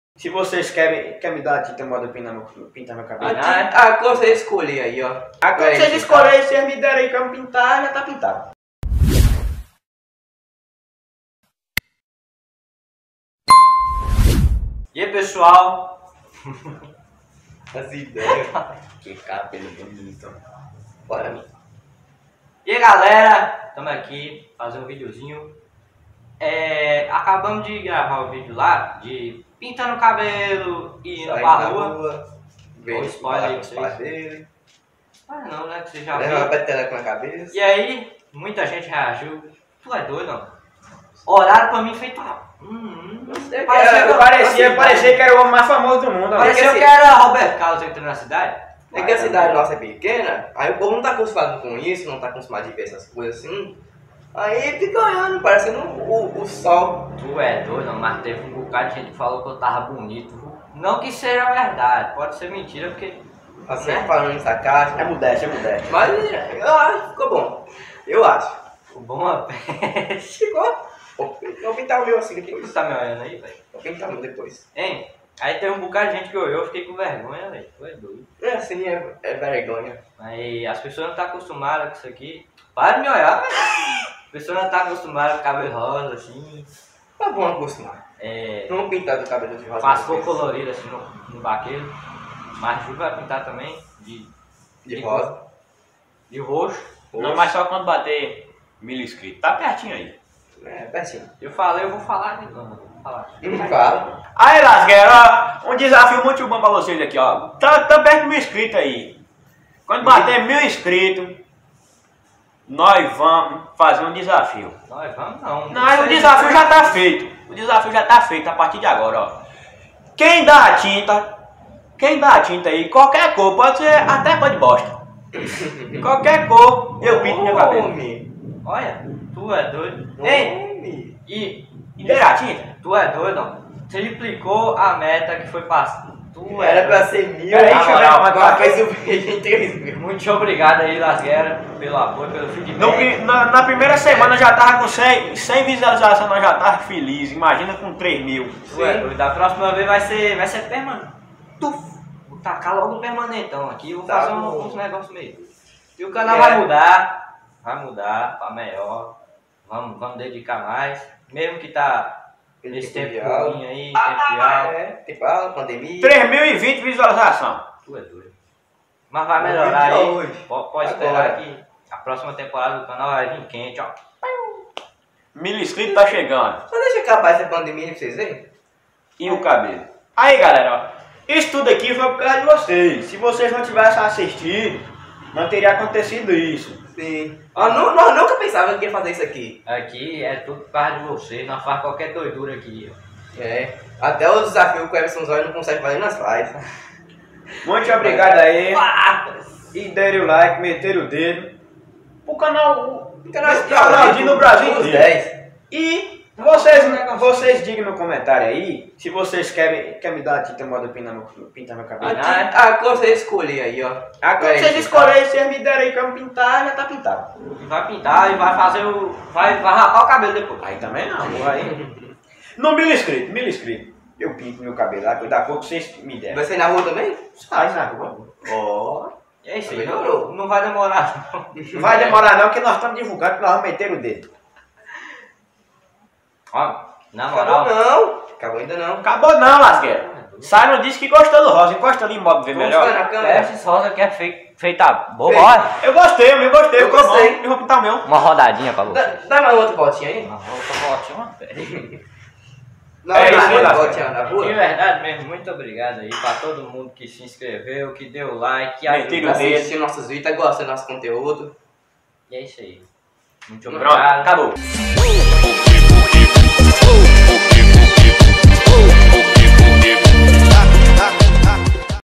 Comentando sobre o vídeo anterior e novo DESAFIO!!! ‹ NunSei ›
Eai meus consagrados beleza!?
O vídeo de hoje é um vídeo simples sem muito corte nem edição, mais para agradecer a vocês pelo apoio que vocês estão dando nos nossos vídeos e tambem para falarmos sobre o novo DESAFIO do canal.